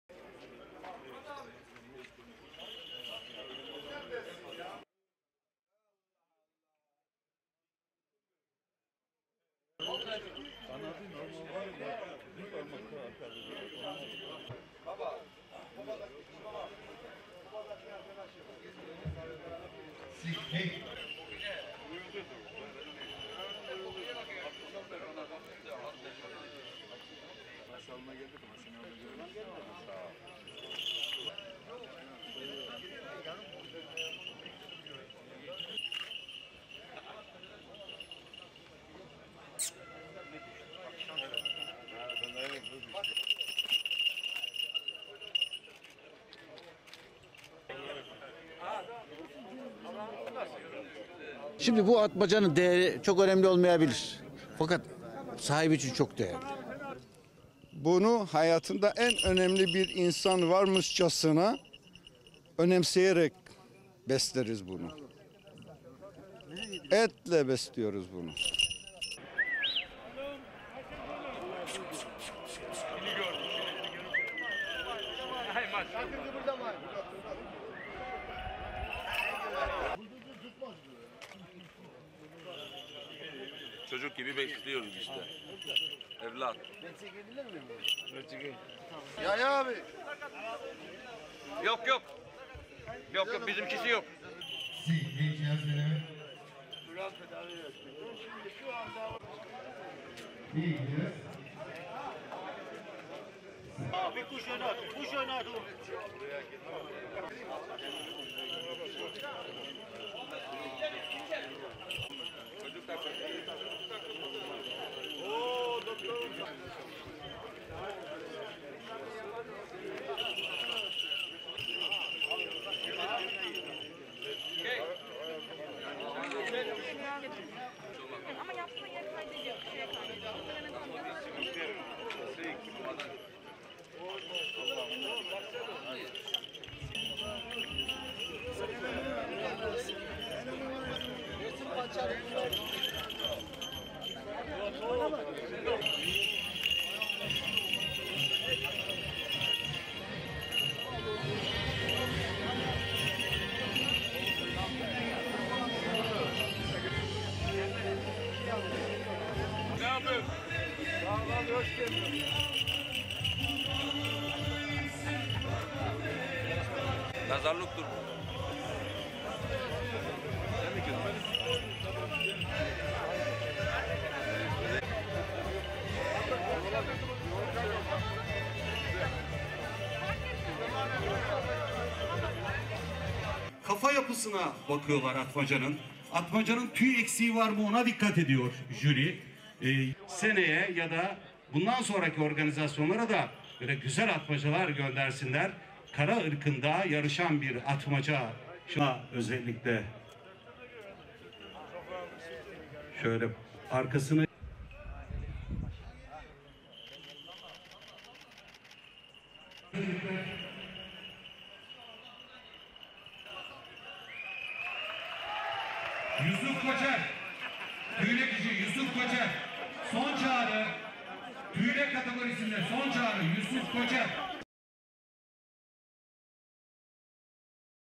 Rize'deki festivalde Şahin Atmacalar güzellikleriyle hayran bıraktı
Vali Erdal Ata Stadyumunda gerçekleştirilen etkinlikte konuşan Ardeşen Belediye Başkanı Avni Kahya, atmaca kültürünün Rize ve Artvin’de yüzyıllardır devam eden bir gelenek olduğunu söyledi.
Kültürün devamı için, festivalin bu yıl ikincisini düzenlediklerini ifade eden Kahya, “Atmaca kültürü çok önemli bir yaşam biçimi ve doğa koruma şeklidir. Atmaca tutkunları, hayvanların yaşam alanlarını, doğayı ve suyu korur. Çünkü atmacanın, şahinin ormana, yıkanmak için suya ihtiyacı vardır. Atmaca tutkunları bu işi yaşam biçimi şeklinde yapar” dedi.